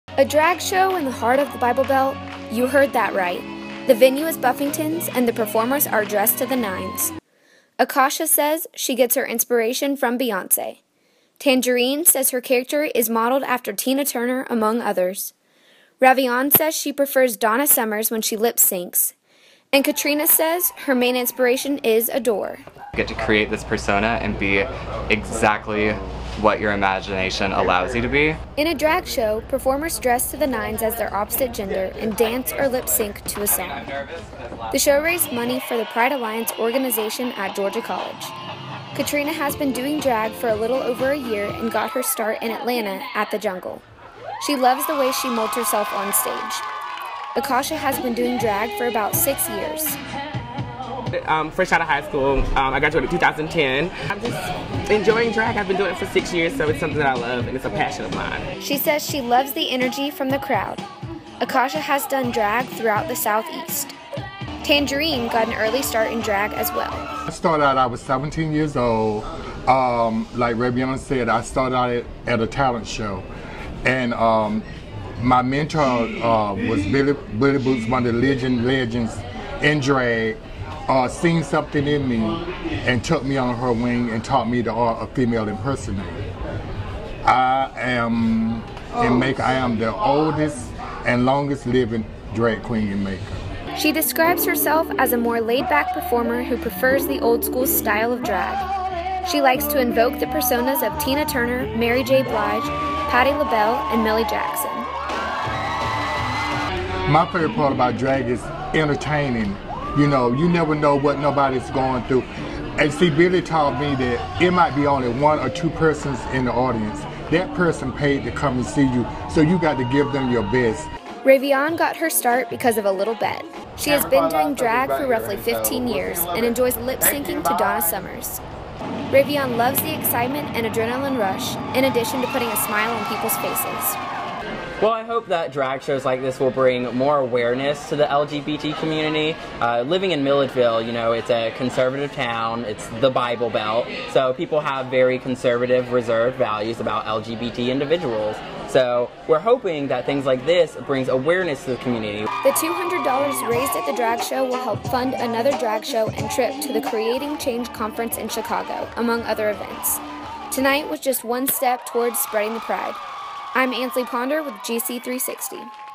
Drag Show
On the Thursday before Thanksgiving Buffington's hosted a drag show, this video has more details!